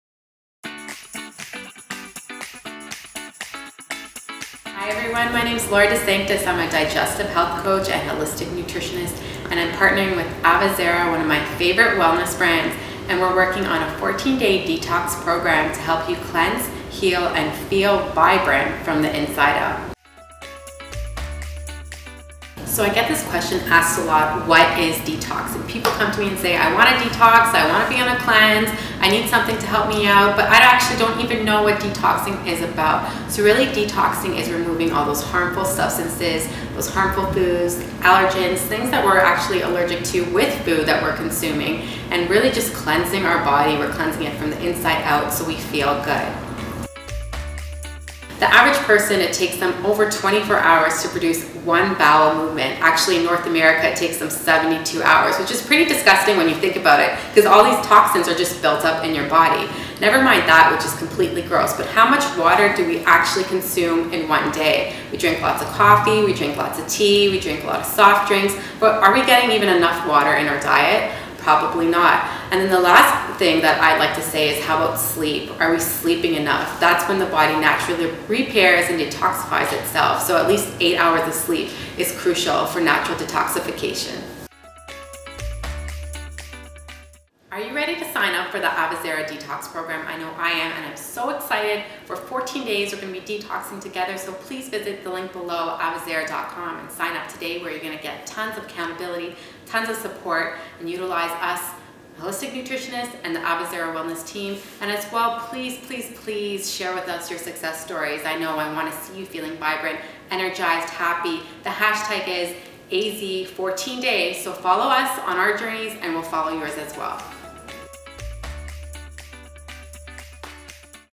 The Avazera 14-Day Detox Program | Meet Laura!
Meet our Holistic Nutritionist, Laura De Sanctis! With The Avazera 14-Day Detox Program Laura explains how you can better your long-term health, way beyond 14 days. Our goal is to empower and motivate you by providing the tools and resources you need to live above a baseline health. This detox program will teach you how to give your body a break from certain foods and common allergens, as well as harmful fats and toxins. Let’s make 2017 your healthiest and happiest year yet, while celebrating the new, vibrant you- together. It’s possible, let us show you how!

It's never too late! to see how you can join The Avazera 14-Day Detox Program!

AVAZERA.COM